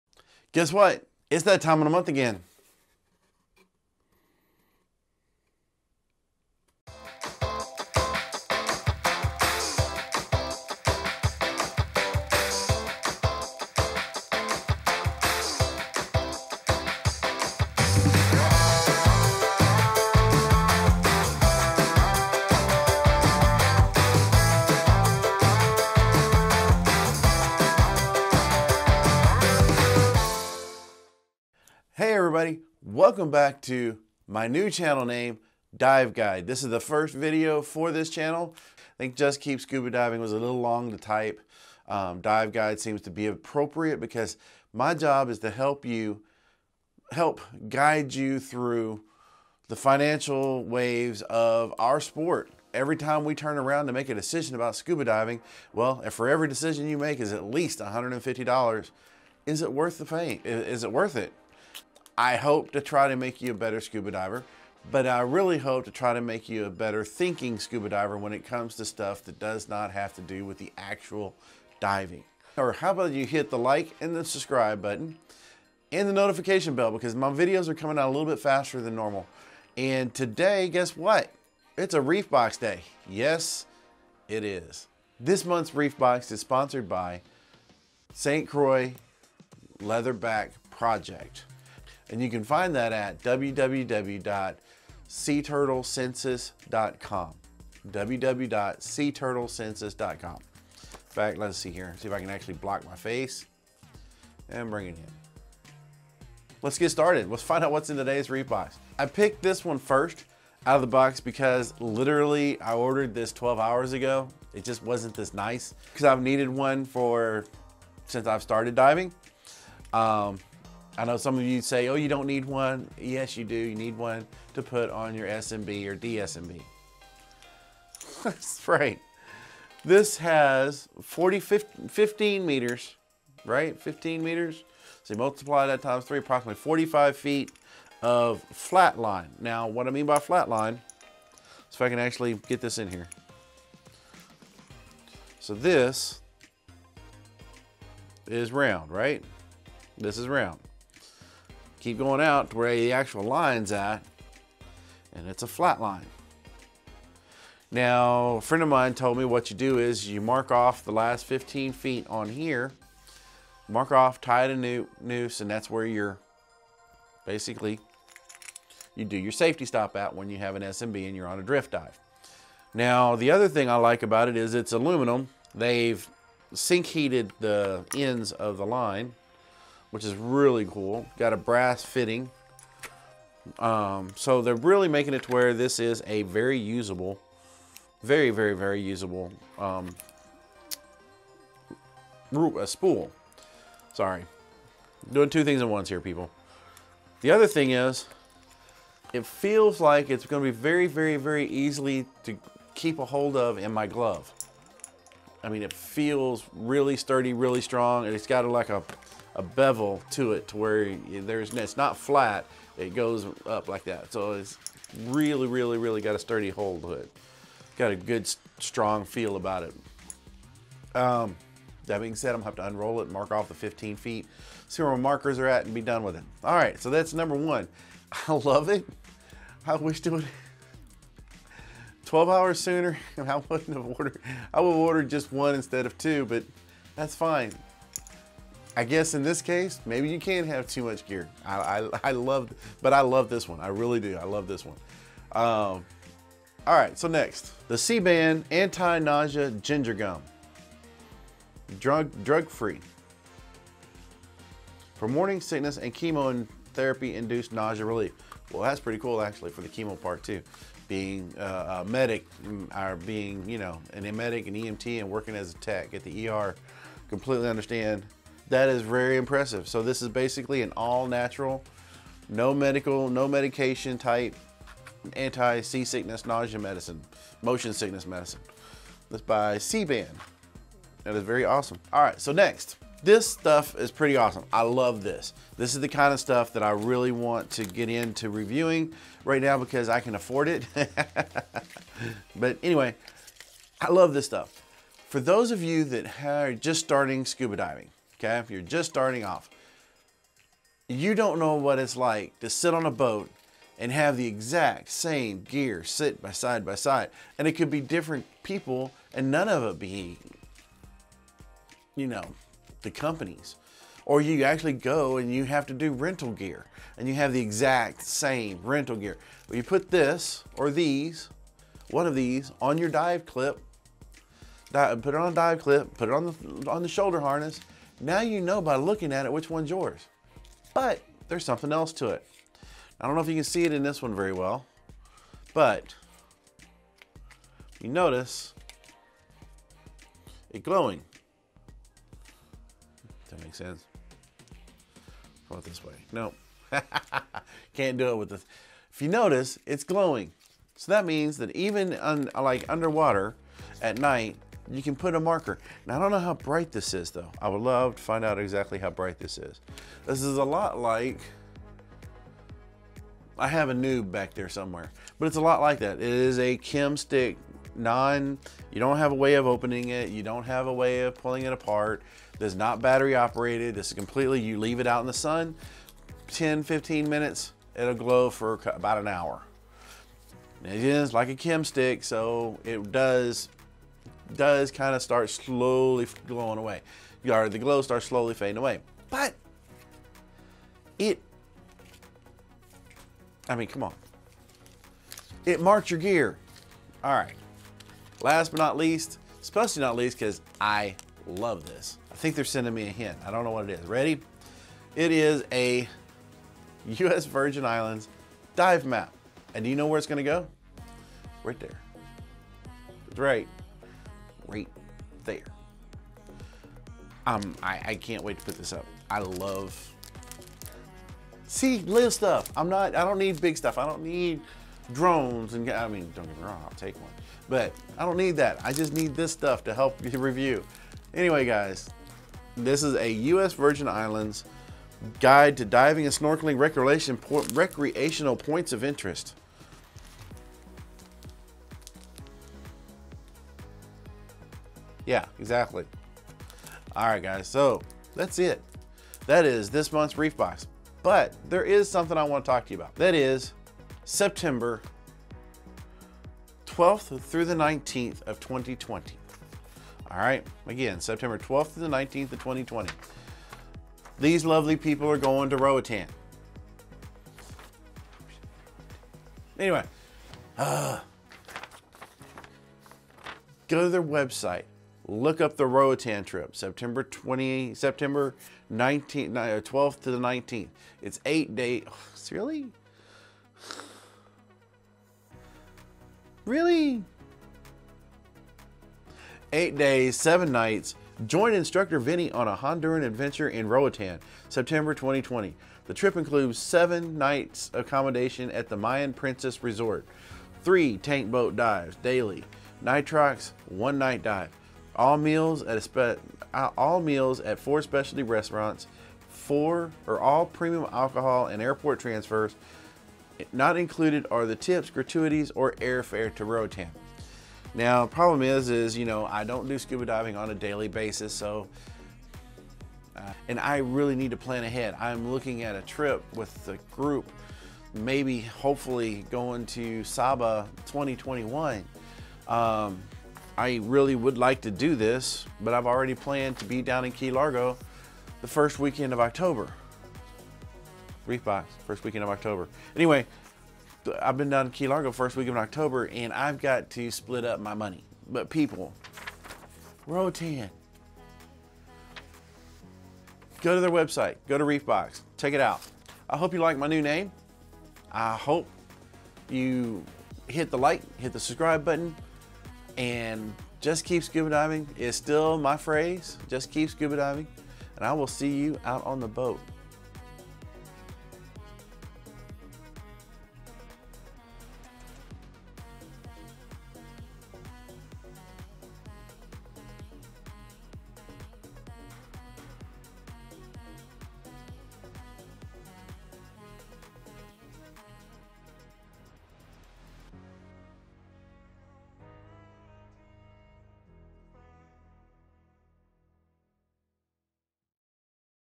Un-boxing Jan. 26, 2020 ReefBox
Hey Fellow Scuba Divers,
We are unboxing January 26th's, 2020, Reef Box. There is a lot of fun small stuff in this box. Including something I have needed since I started scuba diving.
Ther are also a lot of other cool things in this box. But if you really want to see what is available from them head over to their website.

New name on the channel and hopefully a fresh start. Just Keep Scuba Diving and I will see you on the boat.

CHris